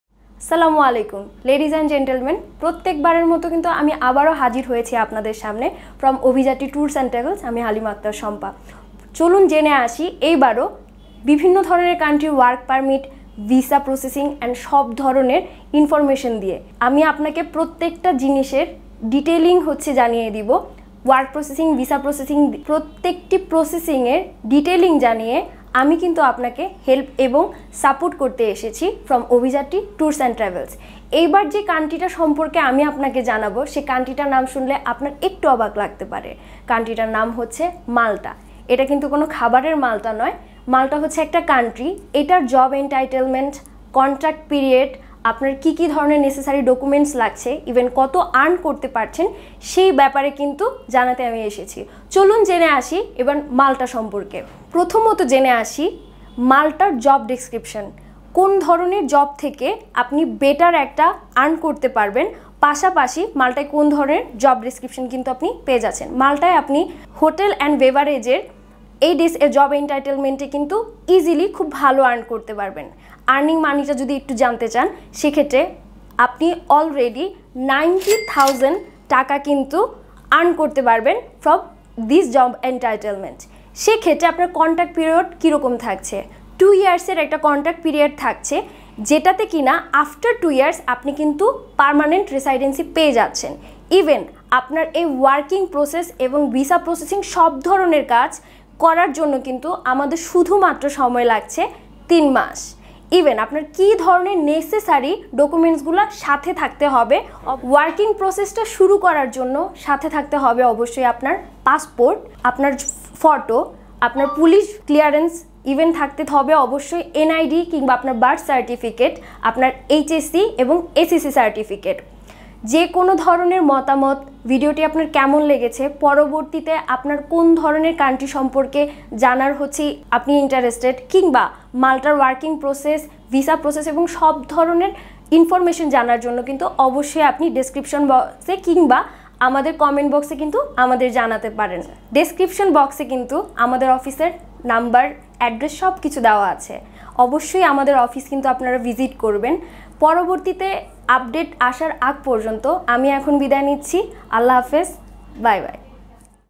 ঢাকায় ফাইল জমা দিন মাল্টার ভিসা নিন -50 হাজার টাকায় | Malta Work Permit Visa!Malta Work Permit
ঢাকায় ফাইল জমা দিন মাল্টার ভিসা নিন-৬০ হাজার টাকায় or Malta Work Permit or Malta Visa or Malta Work Visa or Malta Country are the main topics of this video created by ovizatry.

Ovizatry

ভিএফএস গ্লোভাল বাংলাদেশে ফাইল জমা না নেওয়া পর্যন্ত ভারতের নয়াদিল্লীতে ফাইল জমা দিয়ে ভিসা নিতে হবে।  ফাইল জমা দেওয়ার পূর্বে ঢাকার ভিএফএস গ্লোভাল জমা নিচ্ছে কিনা নিশ্চিত হয়ে নিবেন।